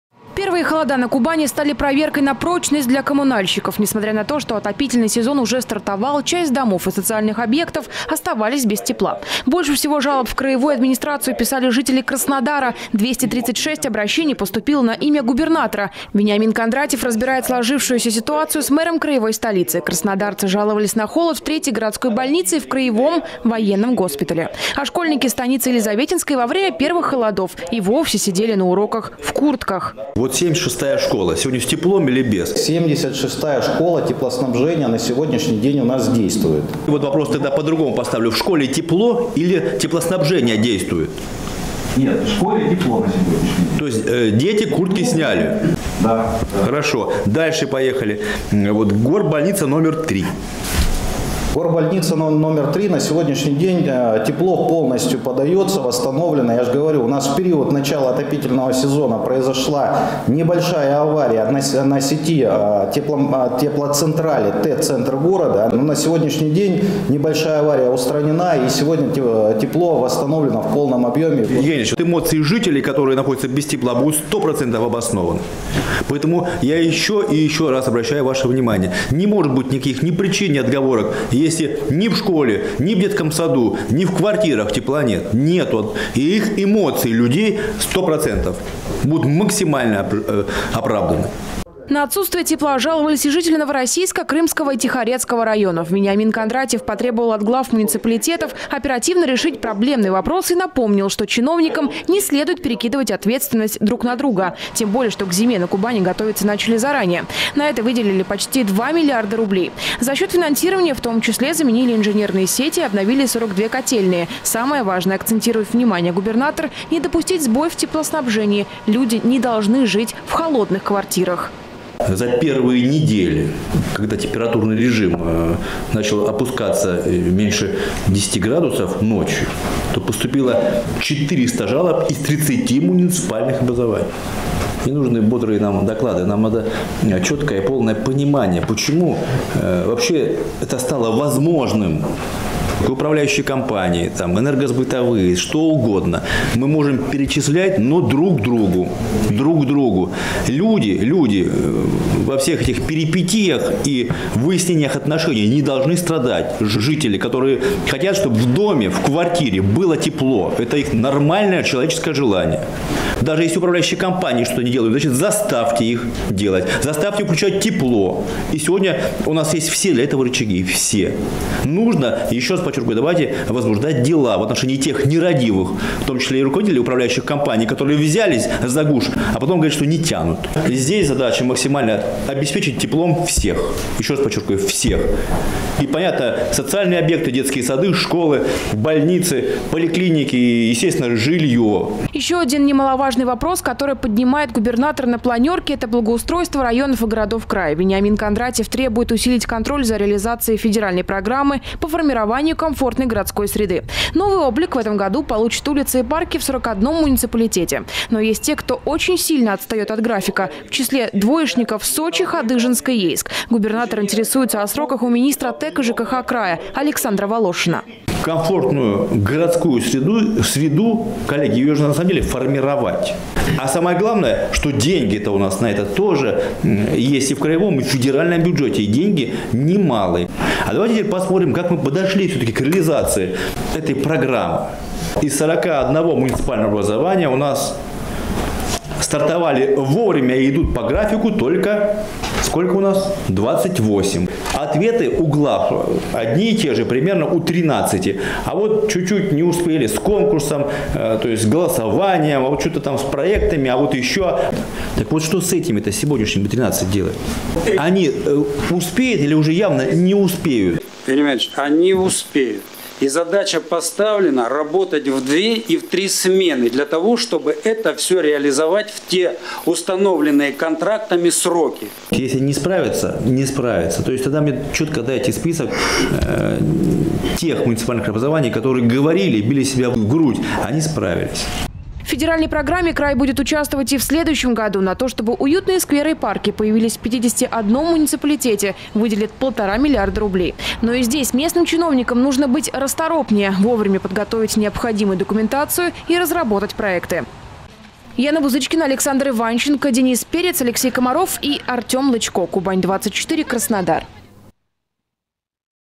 Кондратьев: тепло должно быть во всех домах и соцобъектах. Новости Сочи Эфкате
В краевую администрацию пришло порядка 400-от жалоб. Глава региона поручил главам муниципалитетов проследить за тем, чтобы отопление появилось в каждом доме.